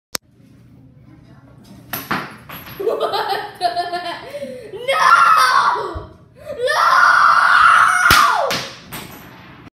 Maniac goes crazy after failed dart trick
We should probably do that again.